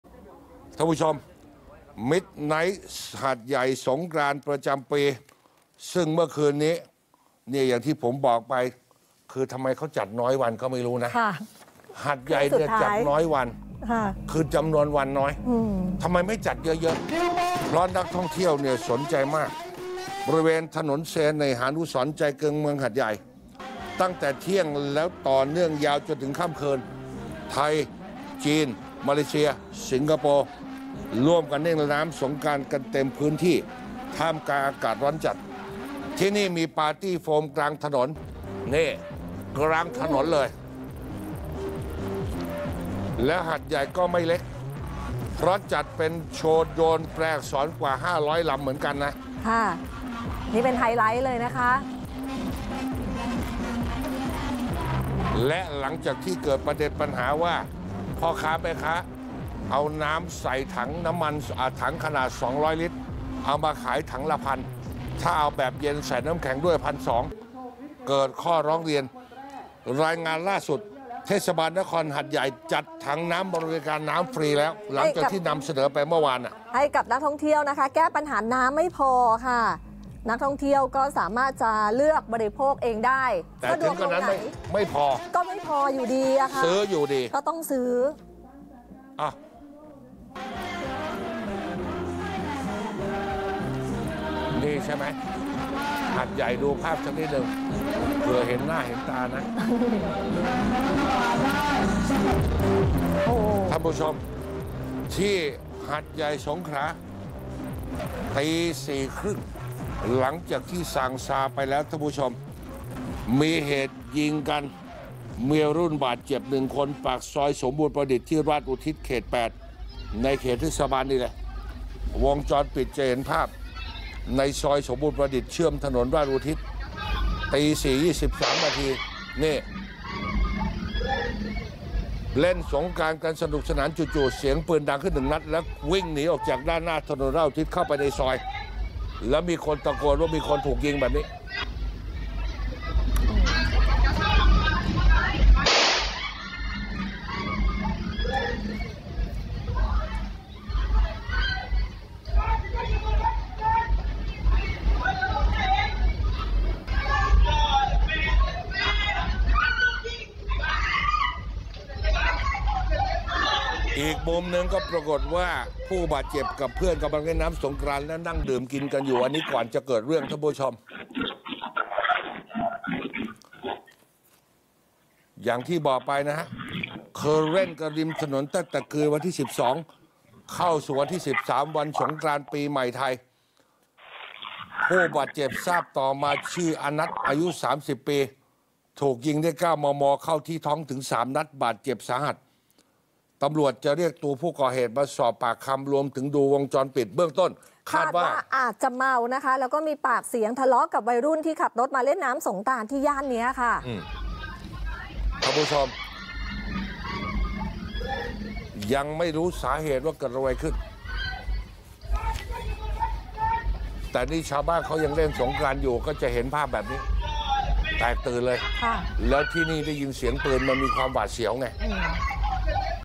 สงกรานต์หาดใหญ่ระทึก ยิงกันเจ็บ 1 ราย นทท.มาเล่นน้ำ วิ่งหนีตายวุ่น
เมื่อช่วงเช้ามืดเวลาประมาณ 04.39 น. วานนี้ (13 เม.ย.) ตำรวจ สภ.หาดใหญ่ จ.สงขลา รับแจ้งเหตุคนร้ายใช้อาวุธปืนยิงกลุ่มวัยรุ่นบาดเจ็บ 1 ราย บริเวณปากซอยสมบูรณ์ประดิษฐ์ ถ.ราษฎร์อุทิศ หรือ เขต 8 ตั้งอยู่ในเขตเทศบาลนครหาดใหญ่ และเป็นถนนอีกสายที่มีการเล่นน้ำสงกรานต์ของเมืองหาดใหญ่

ทั้งนี้ มีภาพจากกล้องวงจรปิดภายในซอยสมบูรณ์ประดิษฐ์ ที่เชื่อมกับ ถ.ราษฎร์อุทิศ ที่เกิดเหตุ ซึ่งเวลาเกิดเหตุอยู่ในช่วงประมาณ 04.23 น. โดยในคลิปเป็นภาพบรรยากาศการเล่นสงกรานต์อย่างสนุกสนาน จู่ๆ มีเสียงปืนดังขึ้น 1 ชุด และมีกลุ่มคนพากันวิ่งหนีจากด้านหน้า ถ.ราษฎร์อุทิศ เข้ามาภายในซอยสมบูรณ์ประดิษฐ์ และยังมีคนวิ่งตะโกนบอกว่า มีคนถูกยิง

ส่วนกล้องวงจรปิดอีกคลิปเป็นภาพขณะผู้บาดเจ็บและกลุ่มเพื่อนกำลังเล่นน้ำสงกรานต์ และนั่งดื่มกินกันอยู่

ที่เกิดเหตุพบมีการวางเต้นท์เล่นสงกรานต์ อยู่ริมถนนตั้งแต่เมื่อคืนวันที่ 12 เม.ย.ที่ผ่านมา โดยมีการตั้งโต๊ะ เก้าอี้ และมีเหล้า เบียร์ รวมทั้งของกินวางอยู่ด้วย ส่วนผู้บาดเจ็บทราบในเวลาต่อมาคือ นายอนรรฆ มะเส็ง อายุ 30 ปี ถูกยิงด้วยอาวุธปืน 9 มม. เข้าที่ท้องรวม 3 นัด บาดเจ็บสาหัส และพบปลอกกระสุนปืน 9 มม. ตกอยู่ในที่เกิดเหตุรวม 4 ปลอก จึงเก็บไว้เป็นหลักฐาน โดยคนเจ็บถูกนำตัวส่งโรงพยาบาลหาดใหญ่

ทั้งนี้ ตำรวจจะเรียกตัวผู้ที่อยู่ในเหตุการณ์มาสอบสวนอีกครั้ง รวมทั้งตรวจสอบกล้องวงจรปิดในละแวกเกิดเหตุ เพื่อหาเบาะแสของคนร้ายรายนี้ ซึ่งเบื้องต้นคาดว่า อาจจะมาจากการเมา และเกิดมีปากเสียงทะเลาะวิวาทกับกลุ่มวัยรุ่นที่ขับรถมาเล่นน้ำสงกรานต์ในย่านดังกล่าว

ขณะที่มีข้อมูลจากญาติของผู้บาดเจ็บรายหนึ่ง ทราบว่า ผู้บาดเจ็บออกมาเล่นน้ำ และนั่งดื่มกินกันกลุ่มเพื่อนในย่านเขต 8 แต่ไม่แน่ใจว่า เหตุการณ์นี้เกิดขึ้นได้อย่างไร หรือไปทะเลาะกันเรื่องอะไร

นอกจากนี้ยังมีเบาะแสของผู้ก่อเหตุ ซึ่งเบื้องต้นทราบว่า อาจจะอยู่ในย่านเขต 8 โดยกำลังอยู่ในระหว่างติดตามตัวมาสอบสวน